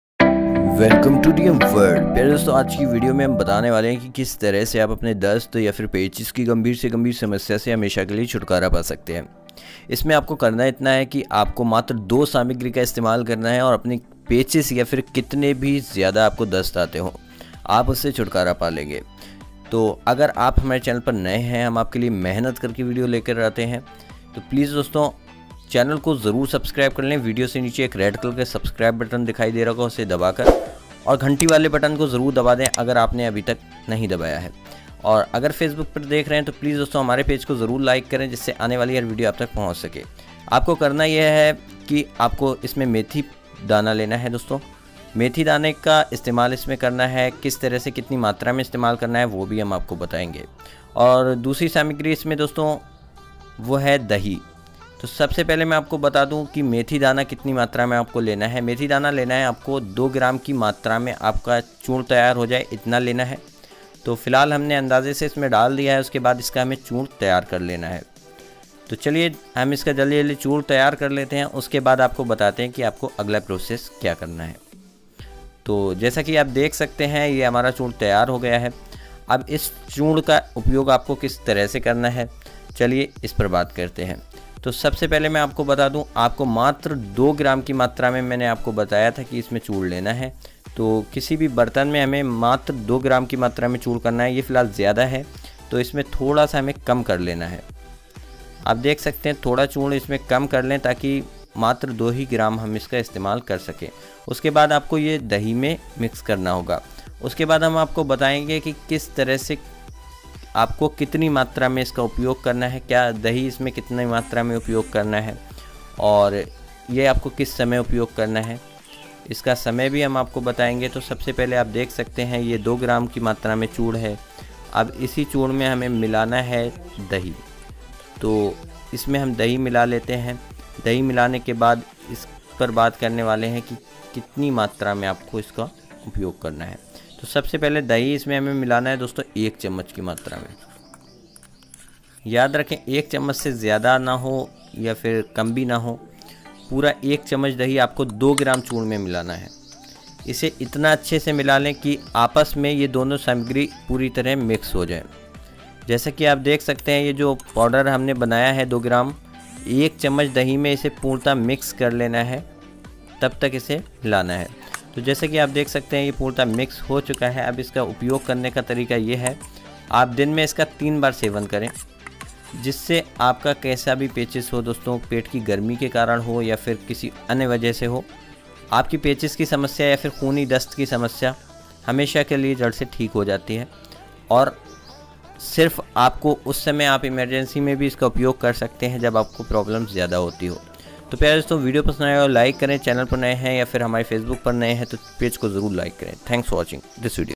पेचिश या खूनी दस्त मात्र 2 ग्राम खा लो तुरंत जड़ से ठीक// khooni Dast or pechis Ka Ramban ilaj
*About Video*

Topic

प्यारे दोस्तों आज की वीडियो में बताया गया है कि खूनी दस्त या फिर पेचिश की समस्या को किस तरह से दूर किया जा सकता है यानी कि उसे किस तरह से छुटकारा पा सकते हैं या फिर आप अगर इस समस्या से जूझ रहे हैं तो इसका आसान तरीका क्या है आप घरेलू सामग्रियों के उपयोग से ही उसके को तैयार कर सकते हैं और बड़ी ही आसानी से अपनी पेचिश या फिर खूनी दस्त की समस्या से हमेशा के लिए छुटकारा पा सकते हैं इसमें आपको करना सिर्फ इतना है कि आपको 2 ग्राम चूर्ण तैयार करना हैऔर दिन में लगभग दो से तीन बार इस्तेमाल करना है जिससे खूनी दस्त या फिर पीछे सब की जड़ से ठीक हो जाती है धन्यवाद !!!

dost video mein bataya Gaya tarika khooni dast ya fir pechis ke liye bahut hi kargar nuskhahai jiska istemal se aapki yah samasya jaldi se theek ho jaati hai ismein mantra 2 samagri ka istemal Kiya gaya hai jo aap ke kitchen mein badi aasani Se mil Jayegi iska upyog Karen aur apni samasya se chutkara pane dhanyvad !!!